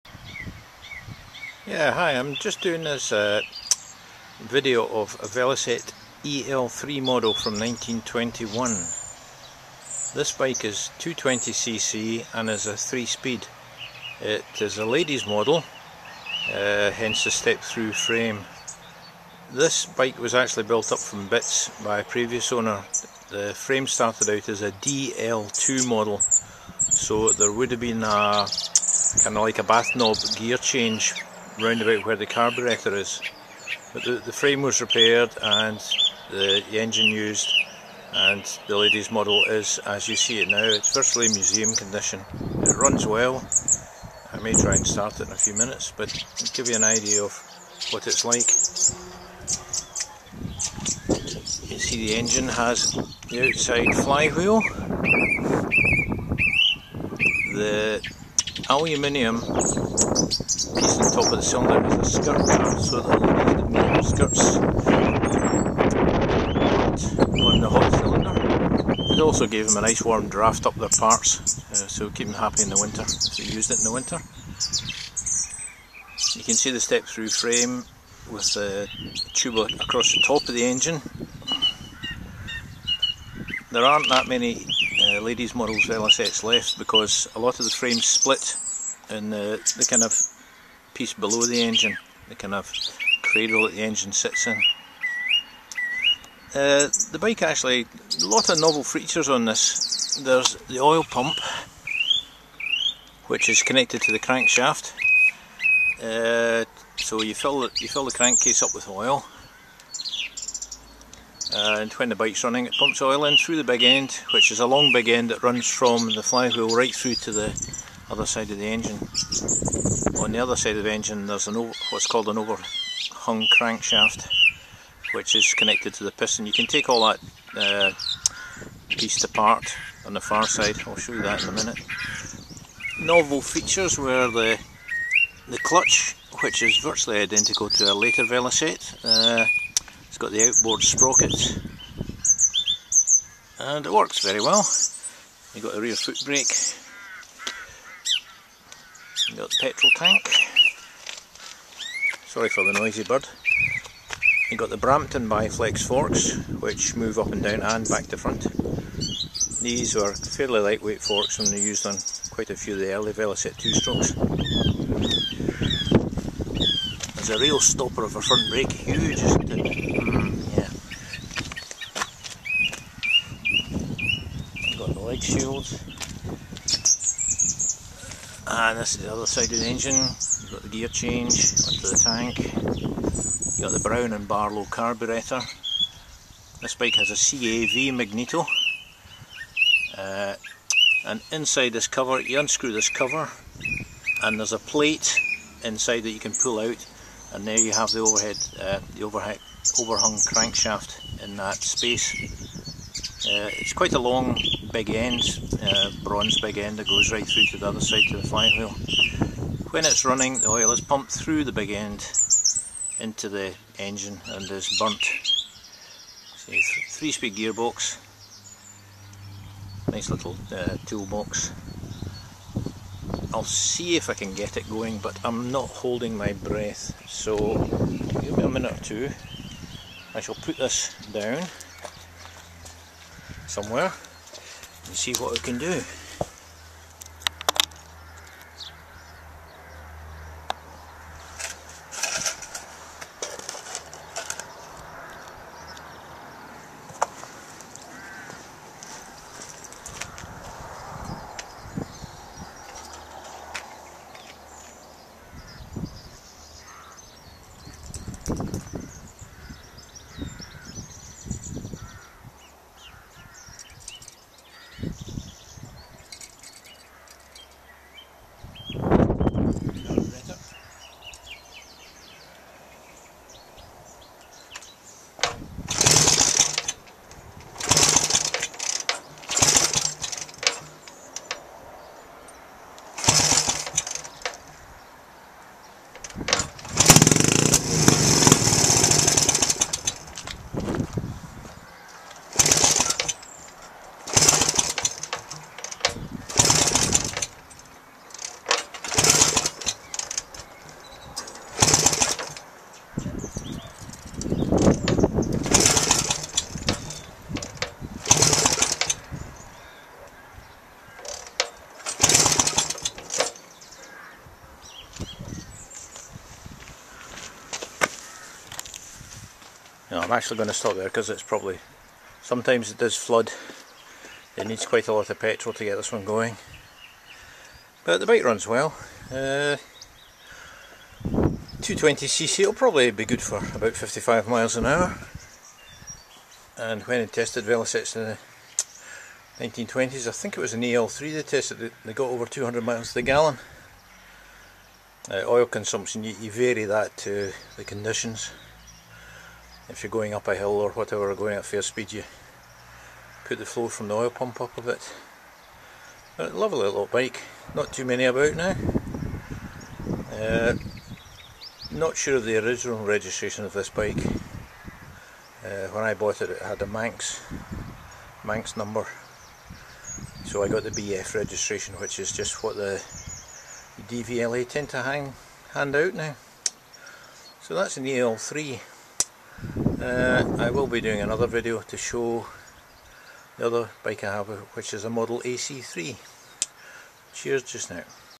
Velocette EL3 1921 - 220cc two stroke
Description as per commentary. 1921, 220cc, two stroke. Was originally a DL2 frame that was modified to an EL configuration and the 3 speed engine fitted. Was in the Island for many years but now in the UK so I had to get an age related registration. As far as I can see there are no other YouTube videos of the early Velocette two strokes. There are a few videos of the GTP model.